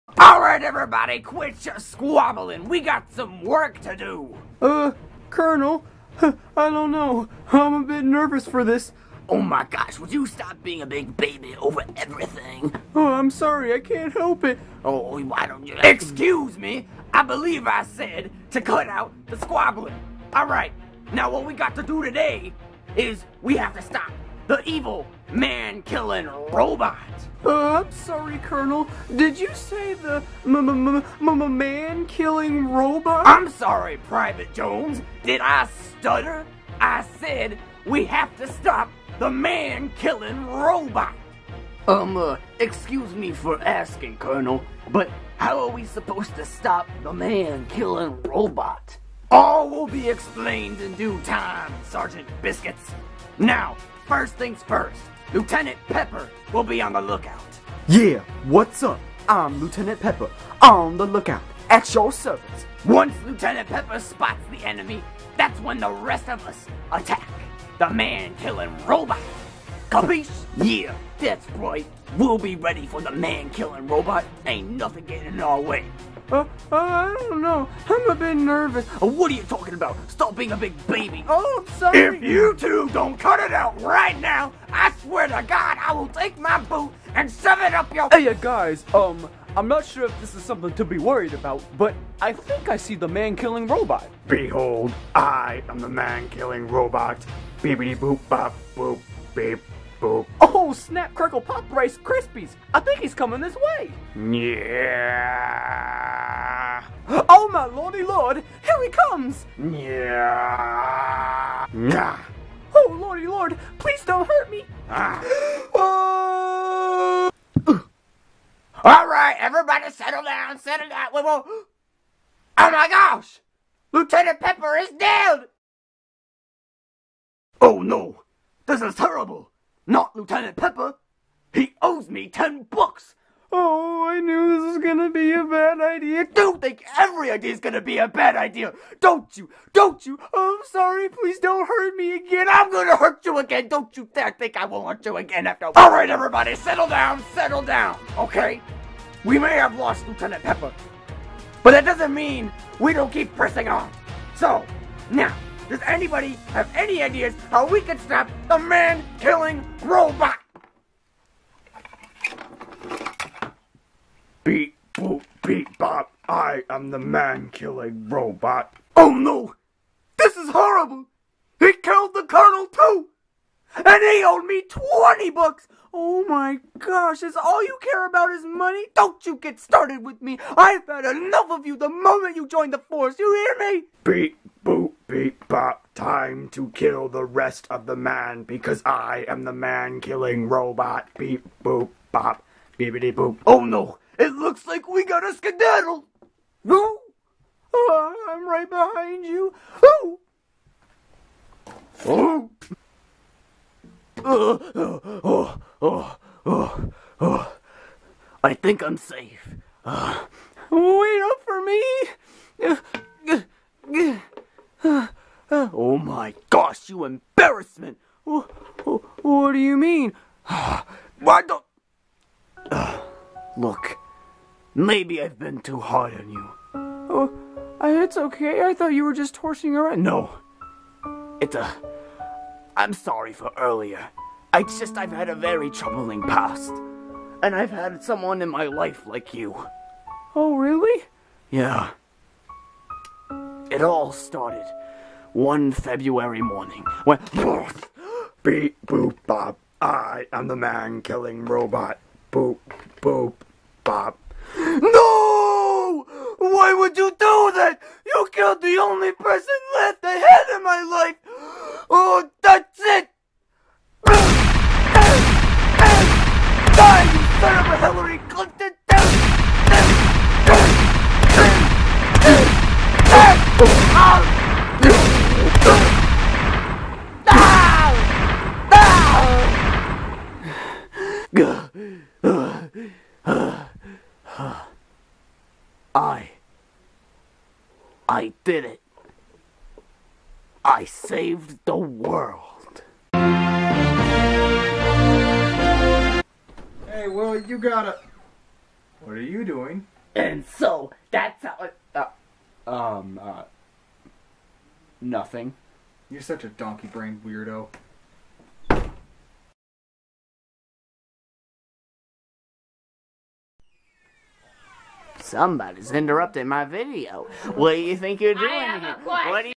Attack of the Man Killing Robot!
When all hope is lost, it is up to the U.S. army to stop our global threat, The Man Killing Robot! But the question remains, will are heroes survive?